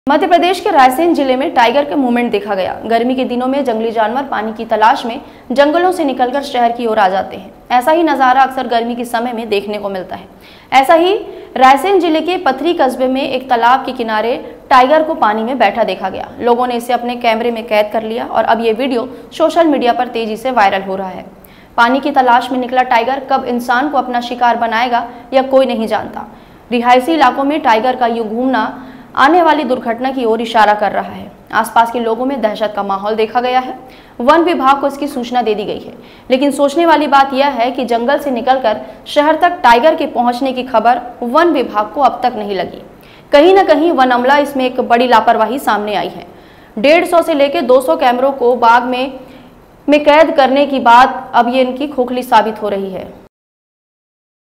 रायसेन : जिला मुख्यालय के पास दिखा टाइगर का मूवमेंट, गर्मी में प्यास बुझाने पहुँचा तालाब
Raftaar Media